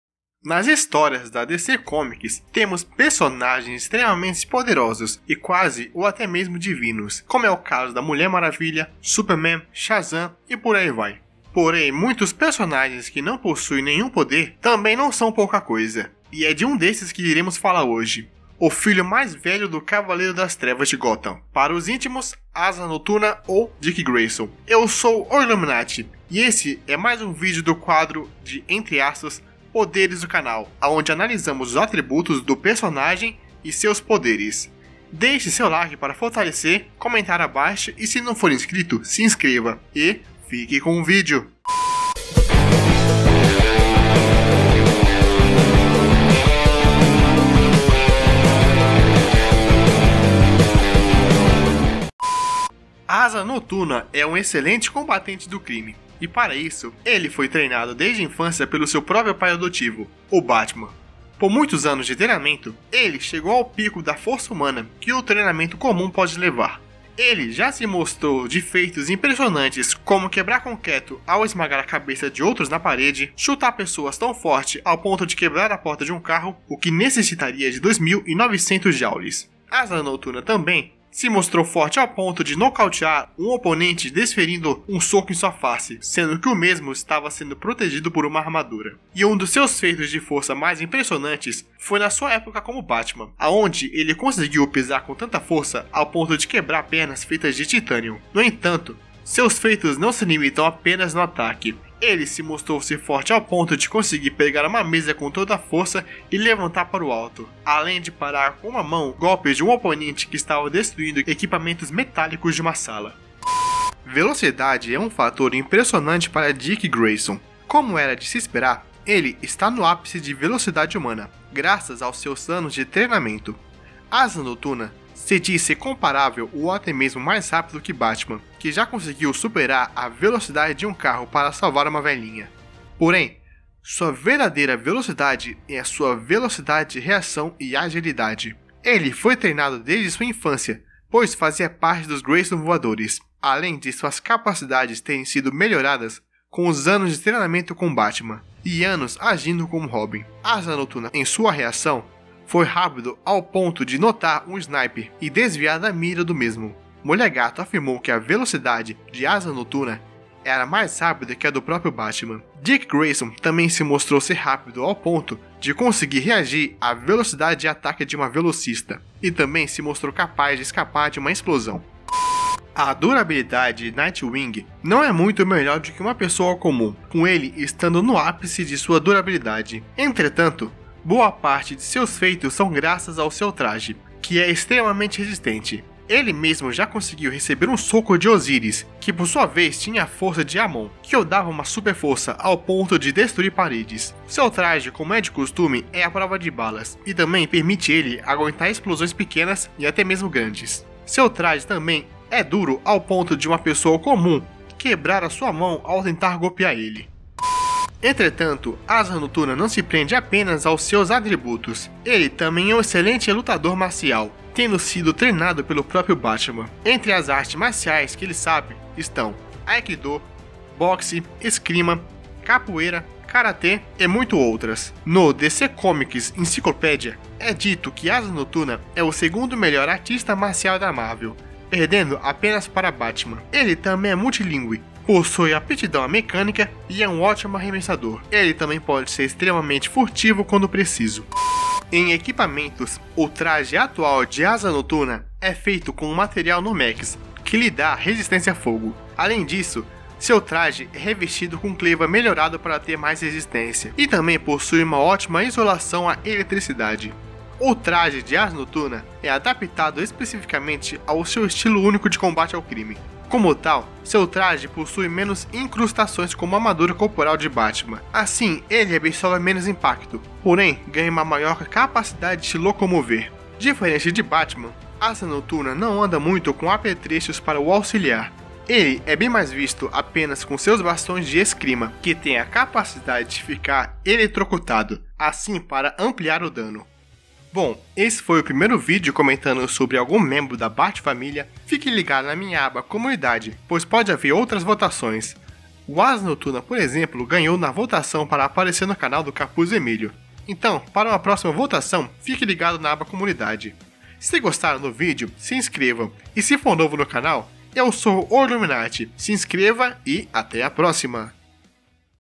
O QUÃO FORTE É ASA NOTURNA? — DC COMICS | O Illuminati
Nas histórias da Dc Comics, temos personagens extremamente poderosos e quase ou até mesmo divinos, como é o caso da Mulher Maravilha, Superman, Shazam e por ai vai, porem, muitos personagens que não possuem nenhum poder, também não são pouca coisa, e é de um desses que iremos falar hoje, o filho mais velho do grande cavaleiro das trevas de Gotham, para os mais íntimos, Asa Nortuna ou Dick Grayson.

😈 NÃO CLIQUE:
📺 Conteúdos do Canal, clique no link encurtado para ser direcionado a paginar de playlist do meu canal